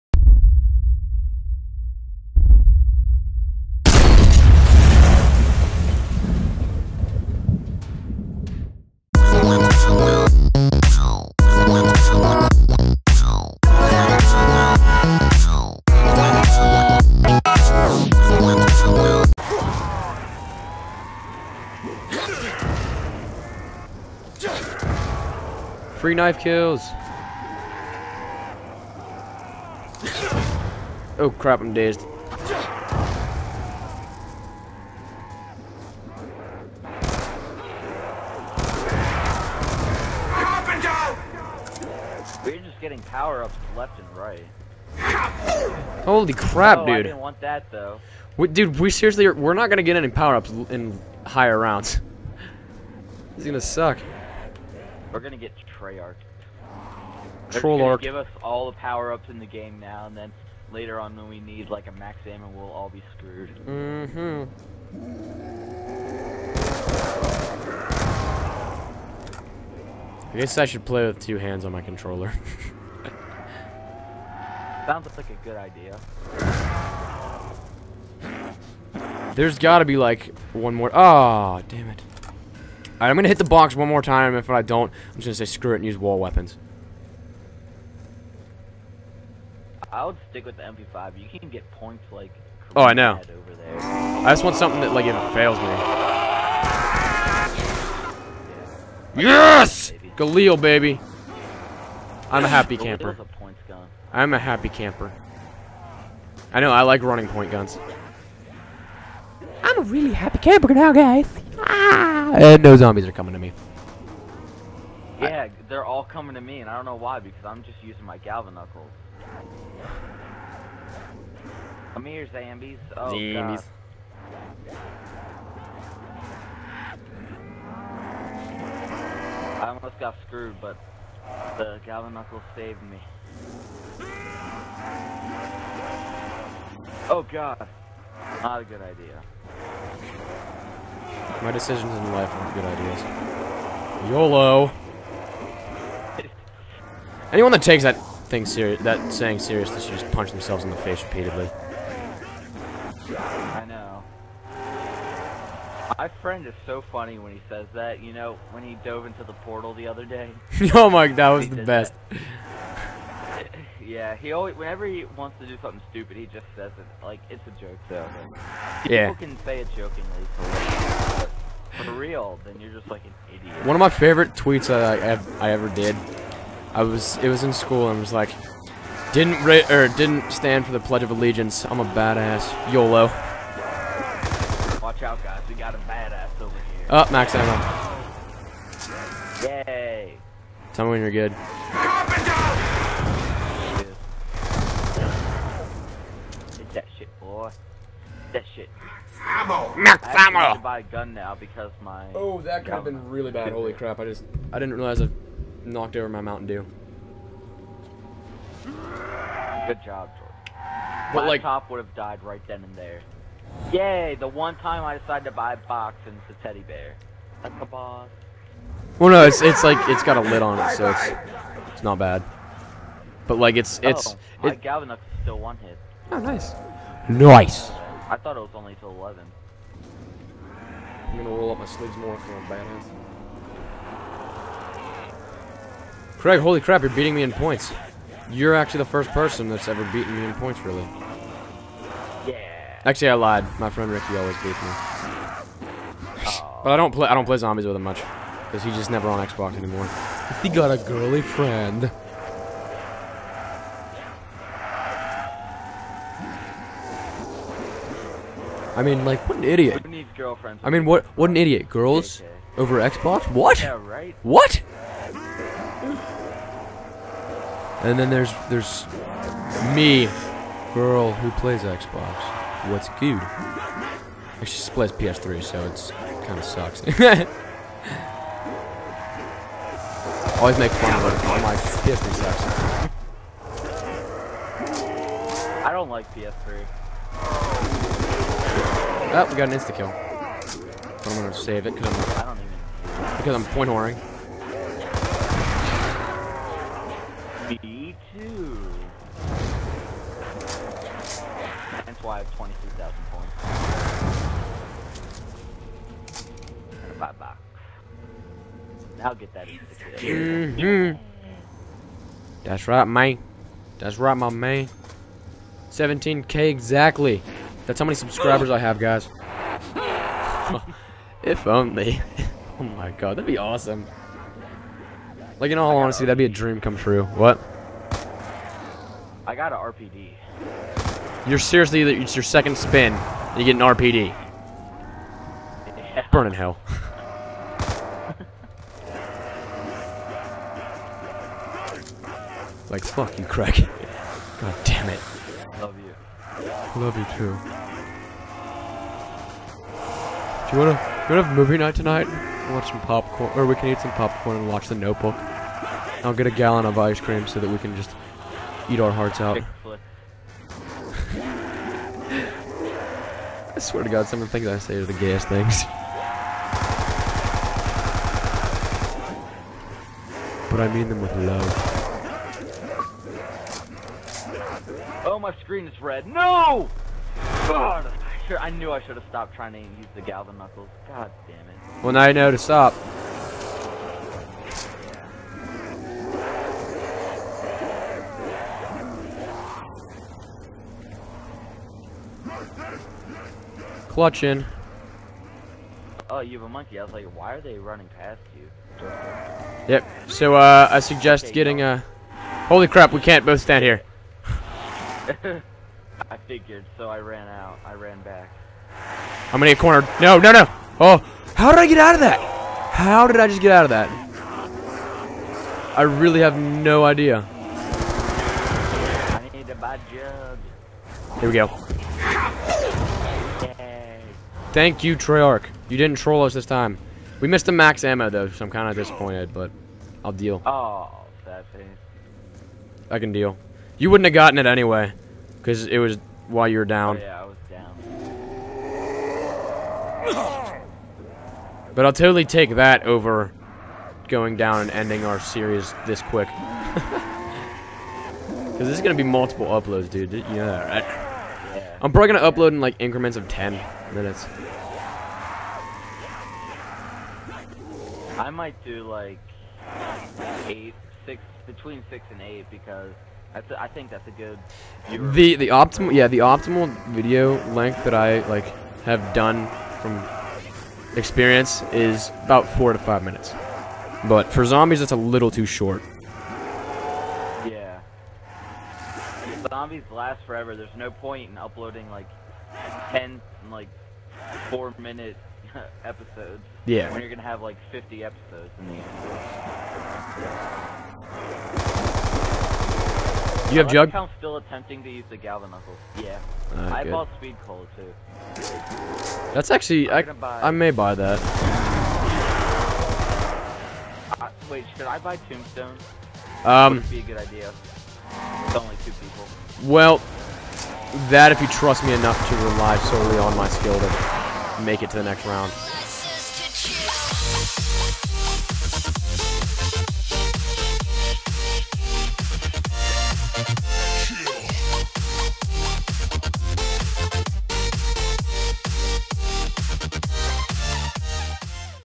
Do You Want to Have Movie Night Tonight? - Zombies w/OperationCraig
Welcome to part three of this live comm series with my sexy friend OperationCraig. My apologies for the late upload and no content for the past couple days. The whole bi-daily thing is still happening, I've just been super busy D:

Be sure to comment, like, and sub. Share the video for a batch of cookiez :D

Be sure to check out Axel Morris, big thanks to him for supplying my outro music!

Til next time guys peace